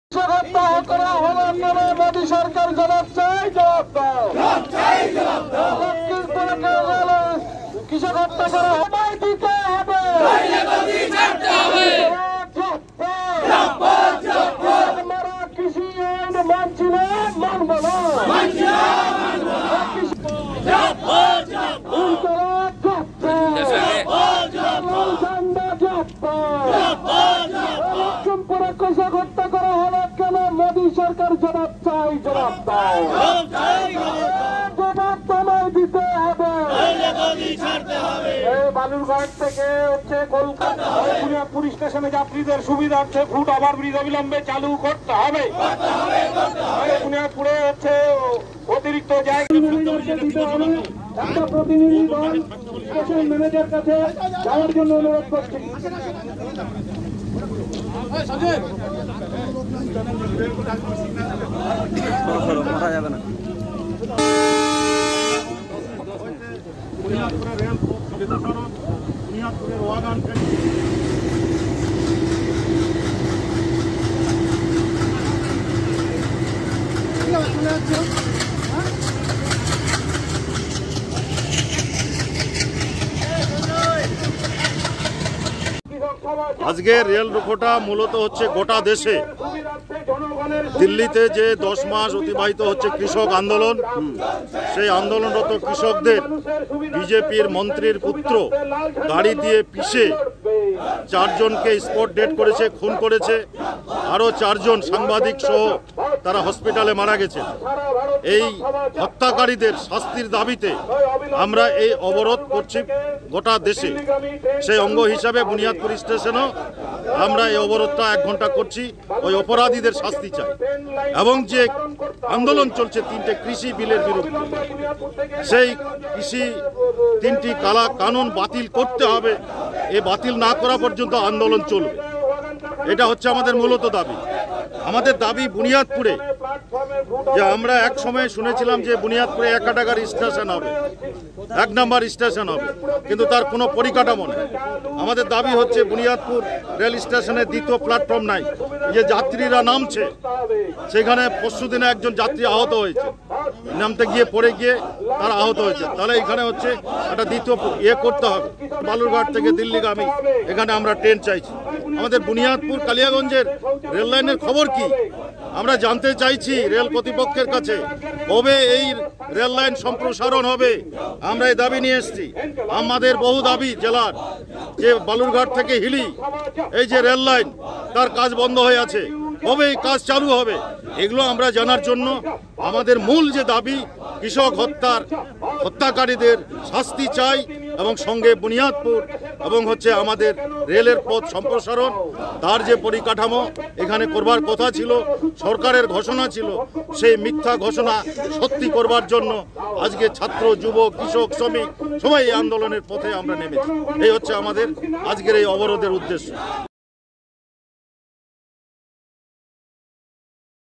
"Rail Roko" in West Bengal : সারা ভারত কিষাণ সভার 'রেল রোকো' অভিযান | Buniadpur 'Rail Roko'
লখিমপুর খেরির ঘটনার প্রতিবাদে এবং কৃষি আইন বাতিলের দাবিতে দক্ষিণ দিনাজপুরের বুনিয়াদপুর স্টেশনে 'রেল রোকো' কর্মসূচি পালন করল বামফ্রন্টের কৃষক সংগঠন সারা ভারত কৃষক সভা । বুনিয়াদপুর স্টেশনে দাঁড়িয়ে পড়ে বেশ কয়েকটি ট্রেন ৷